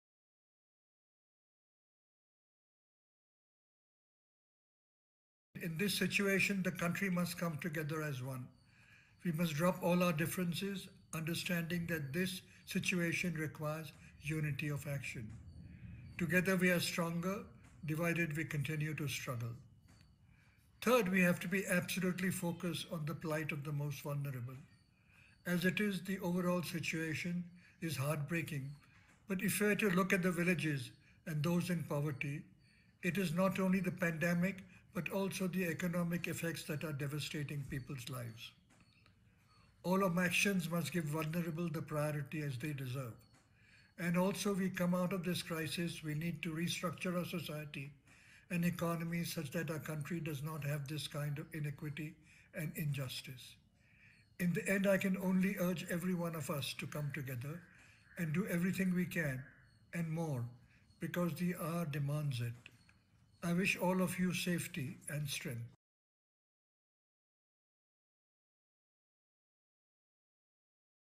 Wipro founder Azim Premji urges all to come together to fight against COVID 19
Source: ANI